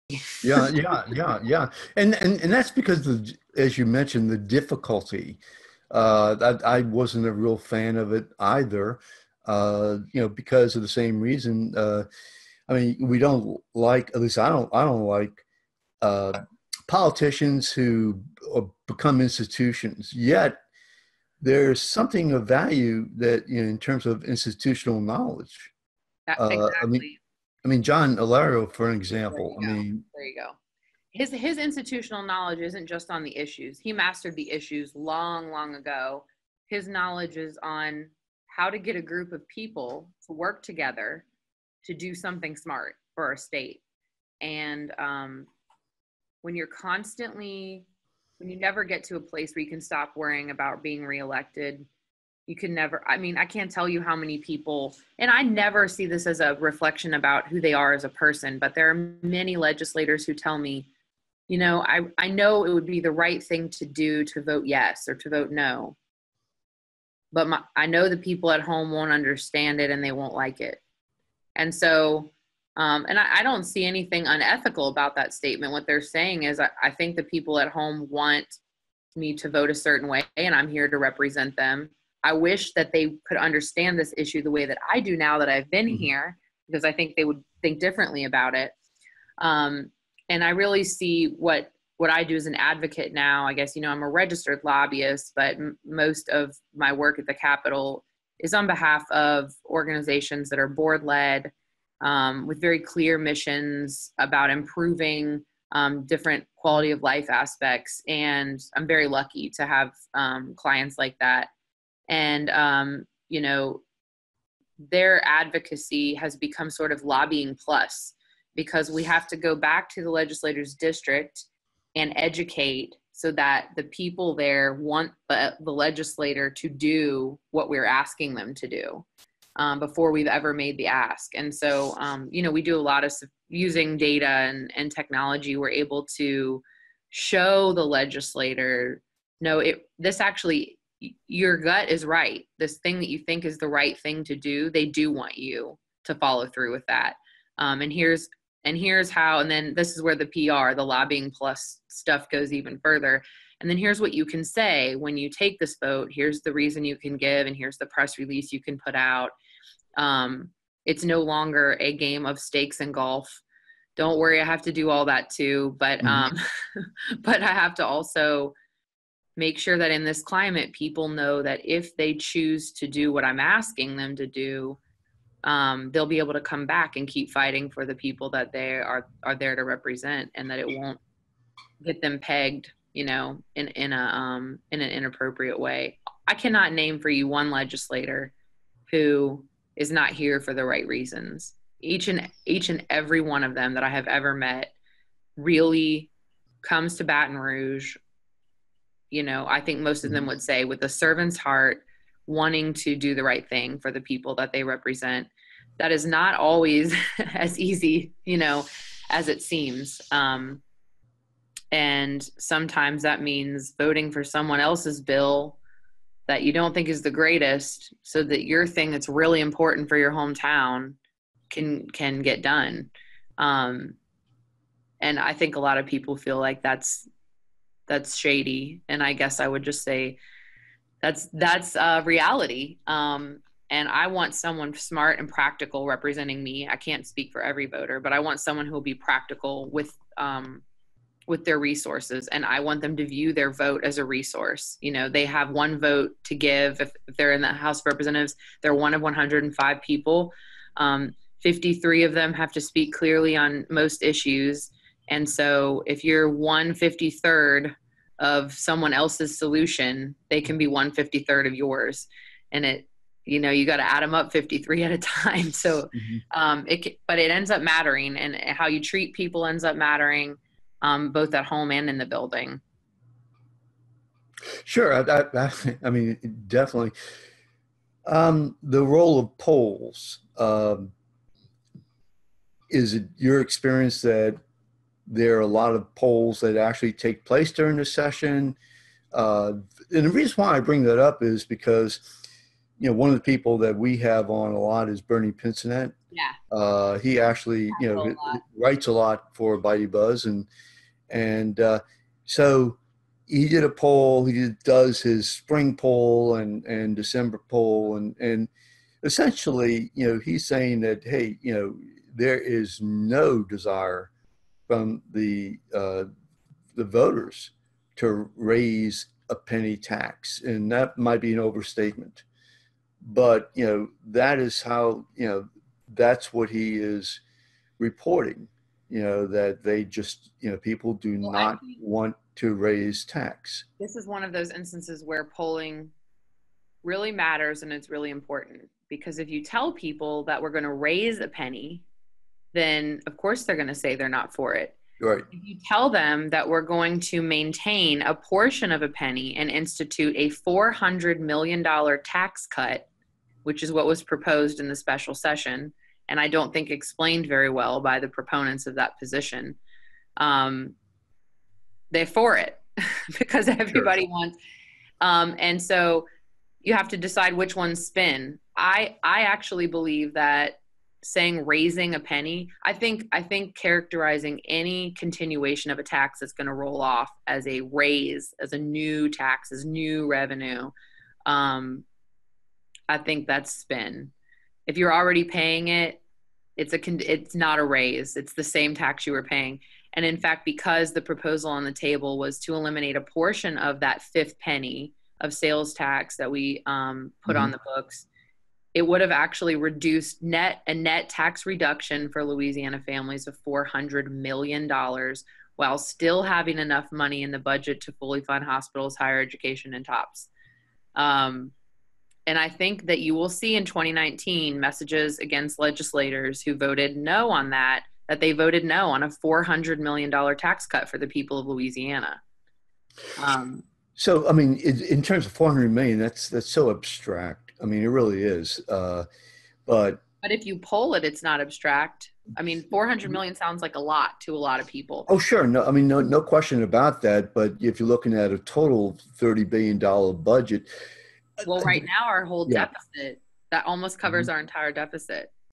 Lobbyist MP Wray discusses John Alario, polls, Louisiana budget deficit
Louisiana legislature elections are approaching. A large segment of those legislators who have served the state will be term limited. Is that program, started by David Vitter when he was a member of the Louisiana House of Representatives, really a good idea, albeit, its popularity?  Do legislators vote their conscience or the the positions of their constituency?
Also, what role does polling play during the Louisiana legislative session?
Last week, we discussed these topicsduring an the interview with lobbyist Mary Patricia Wray.